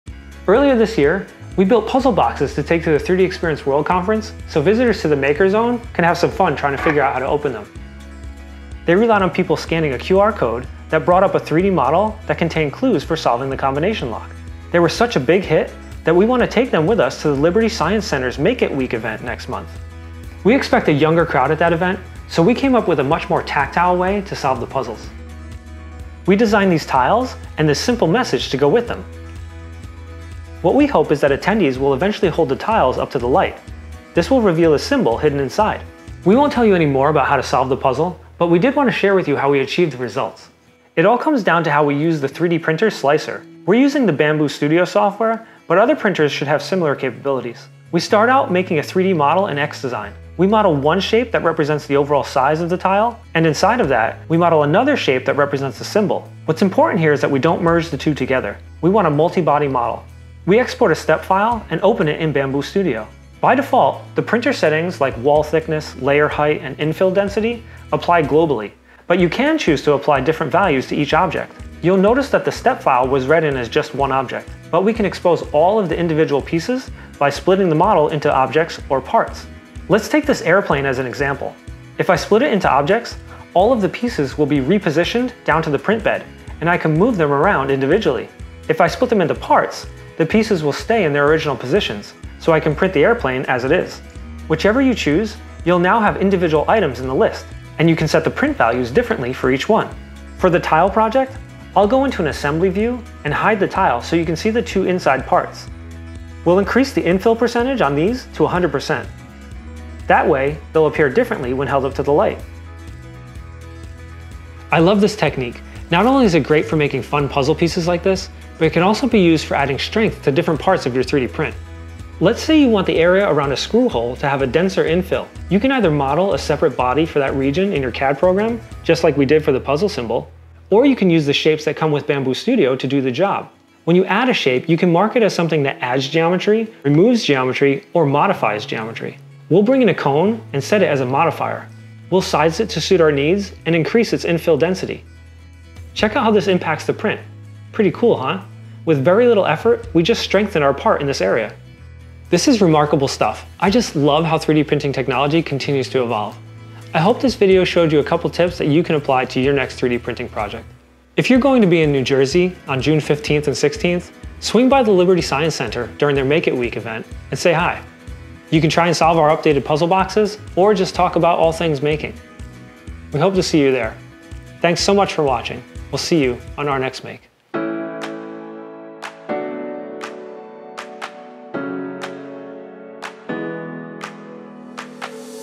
Strengthen your 3D prints with hidden geometry
Using Bambu Studio modifiers to hide secret geometry in your 3D prints to create escape room style puzzles and strengthen your 3D prints.

*We are SOLIDWORKS employees who love to make cool stuff with the software we help to design.